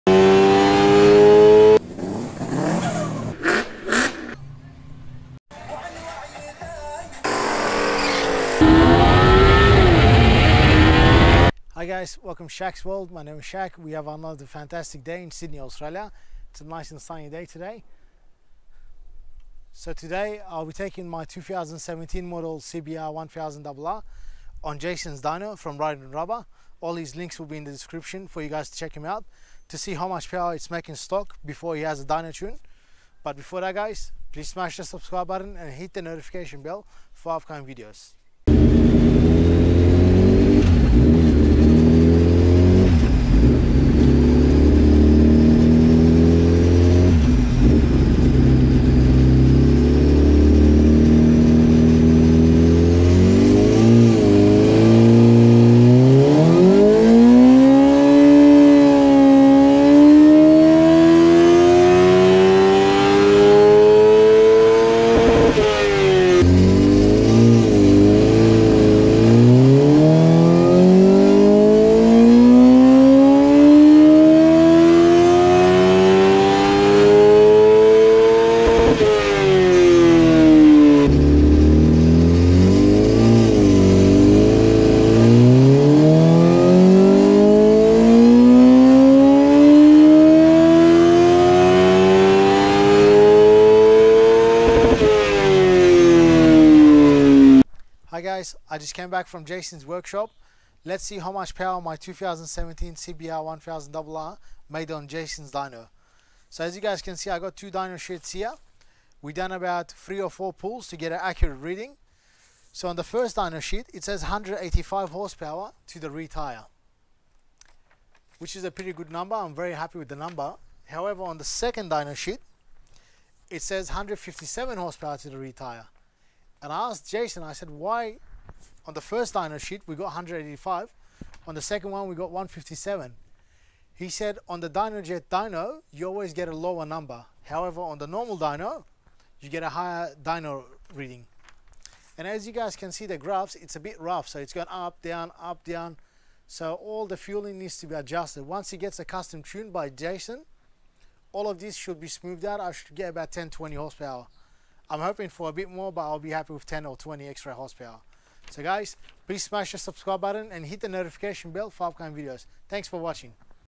2017 Honda Cbr1000rr on Dyno
My stock 2017 Honda Cbr1000rr on the dyno with a arrow slip on